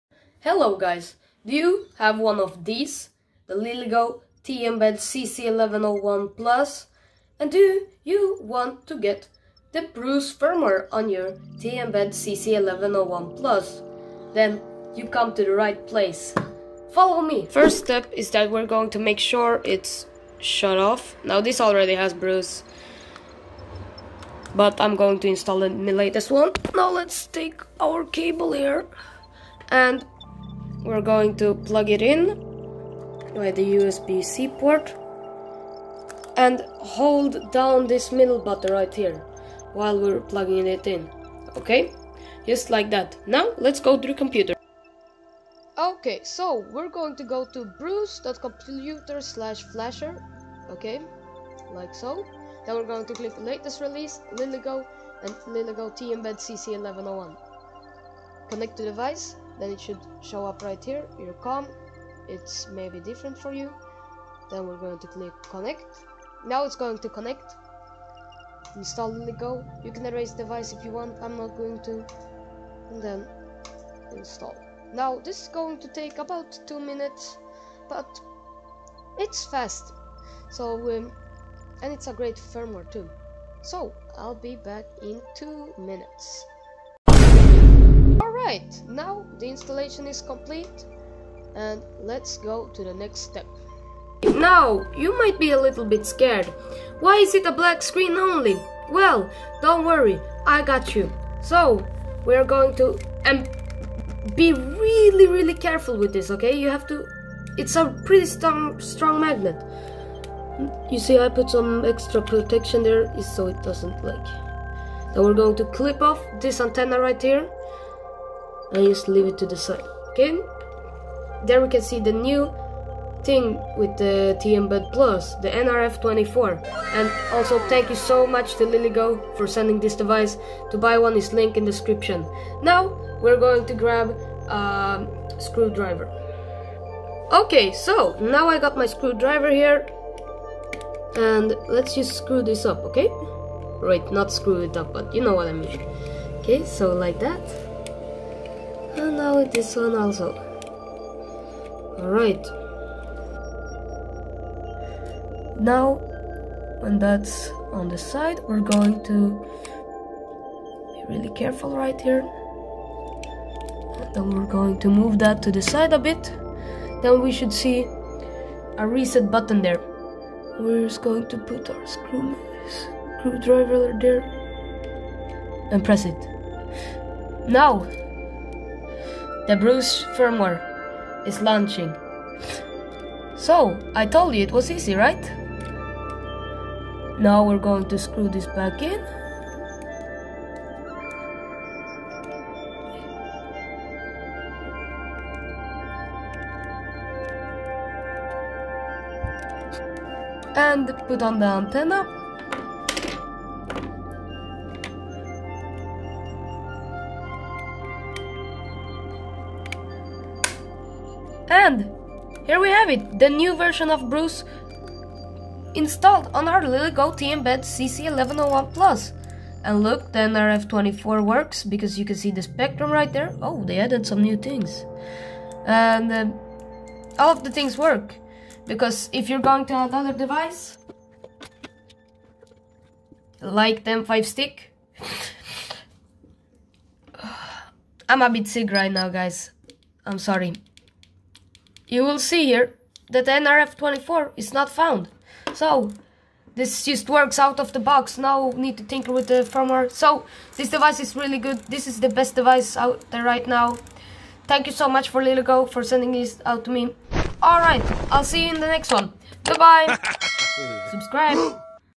How to install BRUCE FIRMWARE on Lilygo T embed cc1101 PLUS!
Hello in this video tutorial im going to show you how to install the bruce firmware on the new lilygo t embed cc1101 plus! The new t embed plus has the nrf24 chip for useful 2.4 ghz exploration!